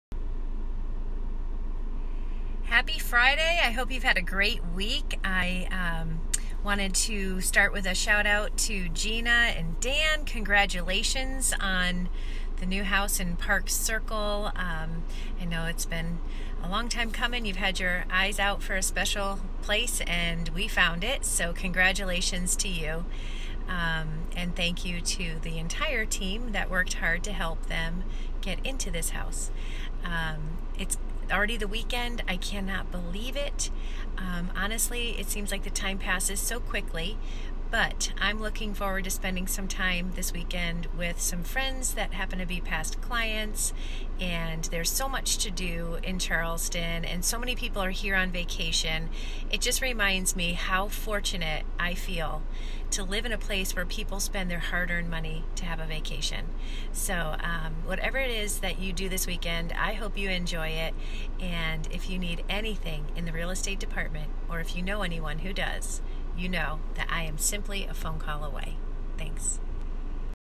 What's Up Charleston July 12
Hello Friday! Wow, it's already the weekend and another busy week here in real estate. Click on my video to hear all my latest updates. If you or anyone that you know is looking to buy, sell or invest in real estate its would be a pleasure to assist.